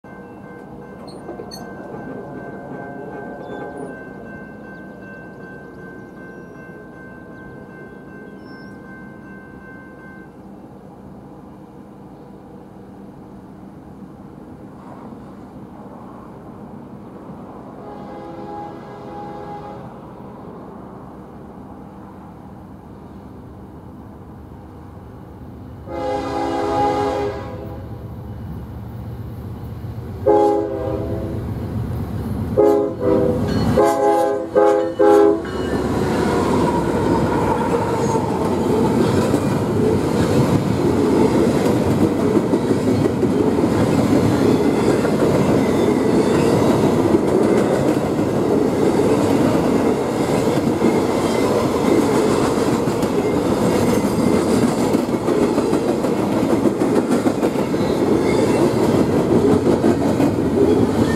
NS 7649 Leads 23G through Butler, IN
NS #7649 ES44DC (Horse Head) 1st Gen K5HL
NS #4176 AC44C6M (Horse Head)